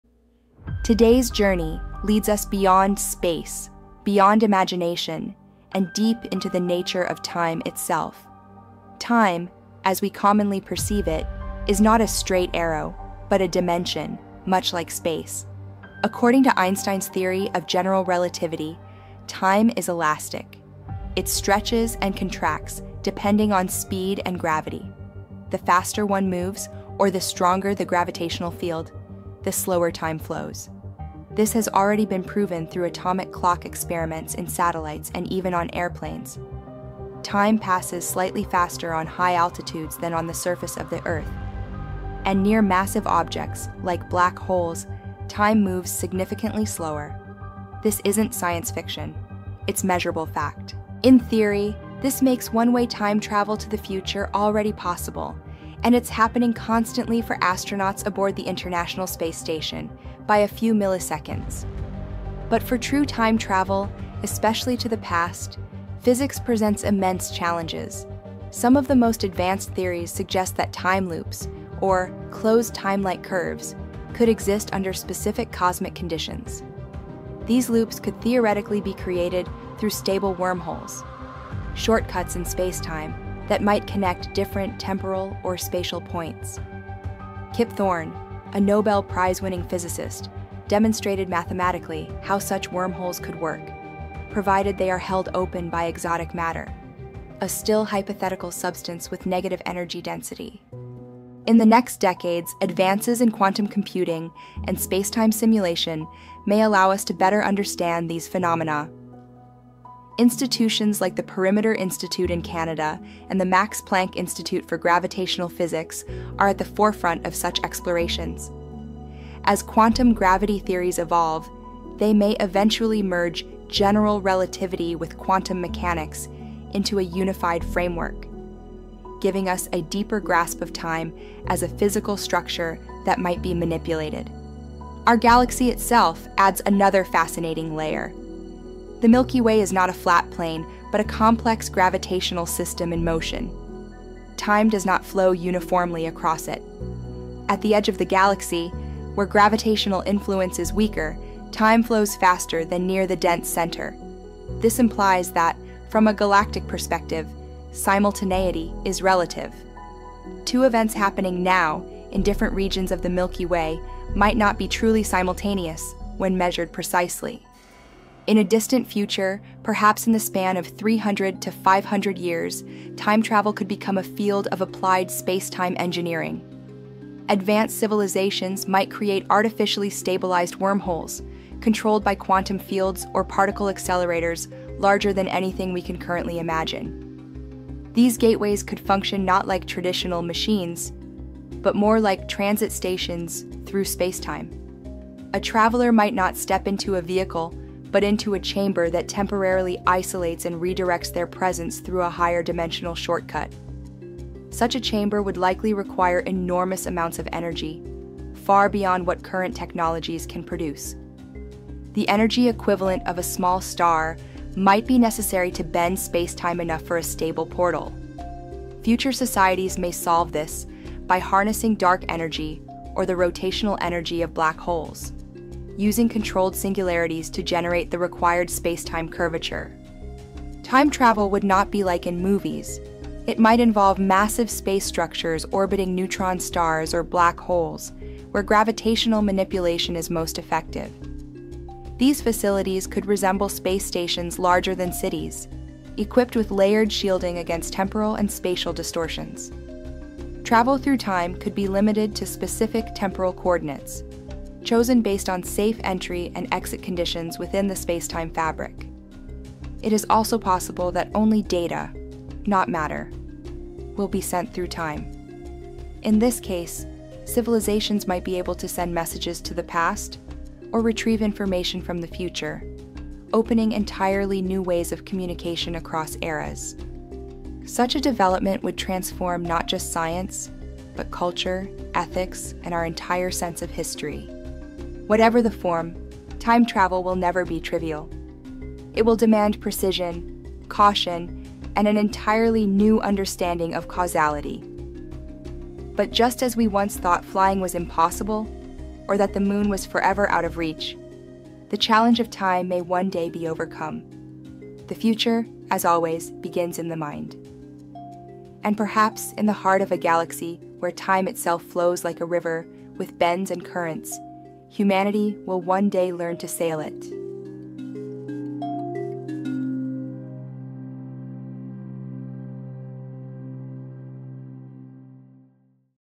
AMAZING WORLD - Time Travel Tech – Science or Just a Dream?
Welcome to a new episode of Amazing World – A Glimpse INTO Our Bright Future
In this journey through science and imagination, we explore the real physics of time. From Einstein’s theory of relativity to wormholes and quantum experiments, this episode dives deep into the mysteries of spacetime – and what it would take to make time travel possible.
Could future civilizations manipulate gravity, stabilize wormholes, and one day build portals through time? What role does our galaxy play in the way time flows?